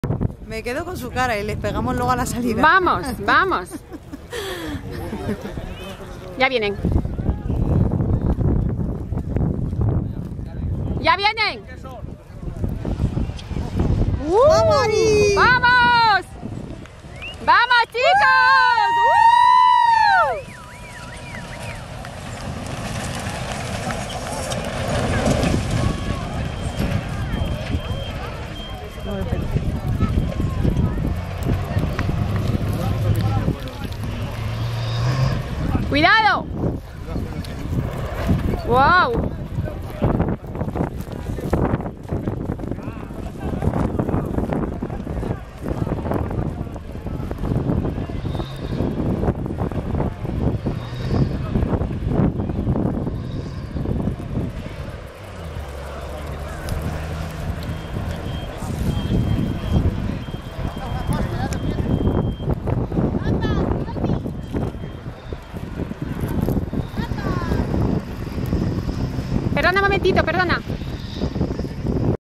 Baja Aragon 2016 Camiones. Equipo truckventur
Paso por control. Estapa 2 , sabado. Zona limitada a 30 kilometros hora.